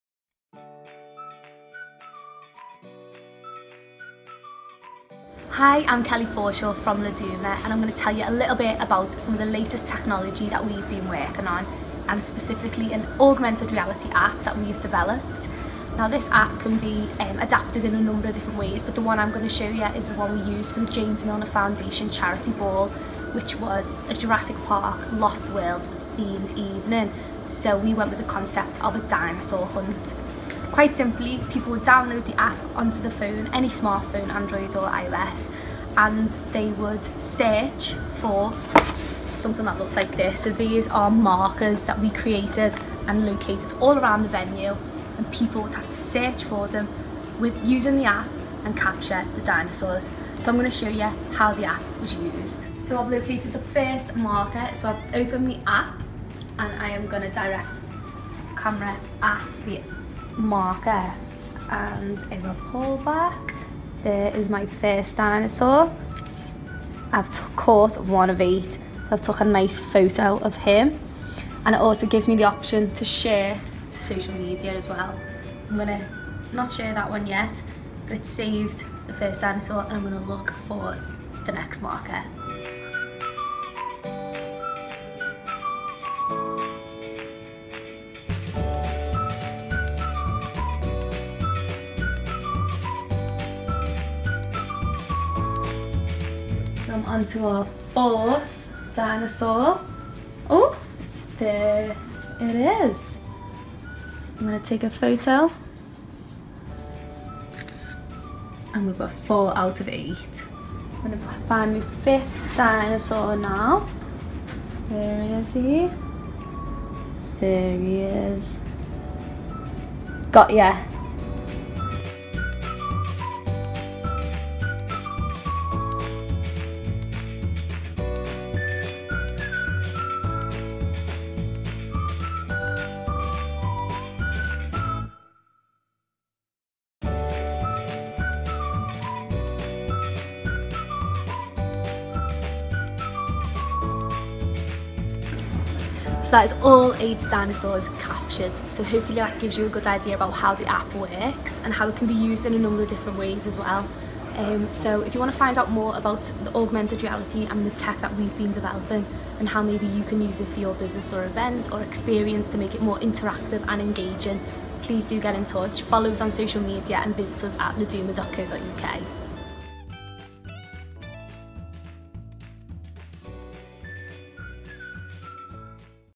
Product demo: Augmented Reality dinosaur hunt app
Laduma's Kelly Forshaw talks through the features of the new augmented reality application, developed by Laduma.
The example app was created for the James Milner Foundation Charity ball where guests partook in a dinosaur hunt to compete for a prize.
The app can be applied and adapted in a number of different industries and themes.